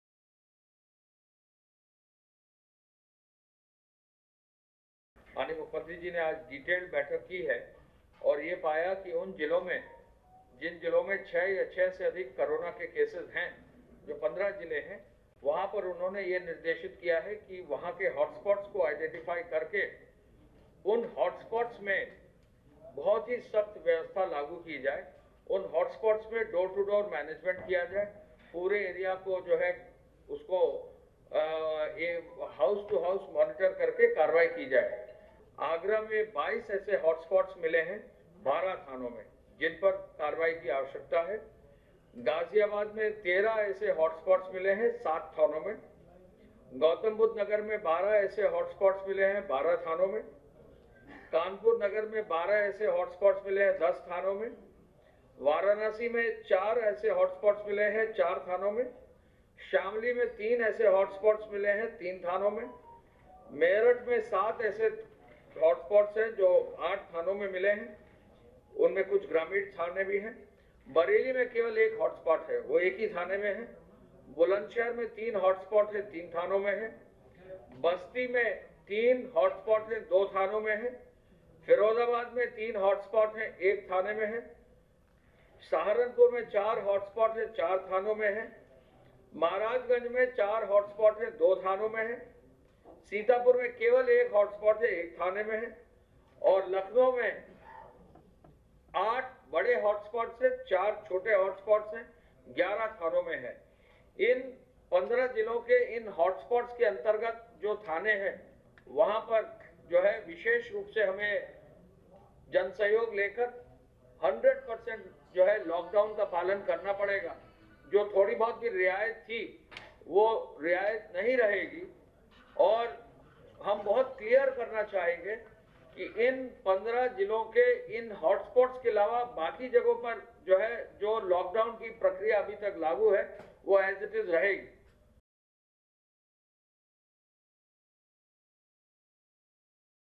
COVID-19: 22 hotspots in Agra, 13 in Ghaziabad, says Avinash Awasthi
Lucknow (UP), Apr 08 (ANI): Uttar Pradesh Additional Chief Secretary (Home) Avinash Awasthi on April 06 informed about the hotspots of COVID-19 in the state. He said, “There are 22 hotspots in Agra,13 in Ghaziabad,12 in Gautam Budh Nagar,12 in Kanpur,4 in Varanasi,3 in Shamli,7 in Meerut,1 in Bareilly,3 in Bulandshahr,3 in Basti,3 in Firozabad,4 in Saharanpur,4 in Maharajganj,1 in Sitapur and 8 big and 4 small in Lucknow.” There have been 343 cases of coronaviryus in Uttar Pradesh.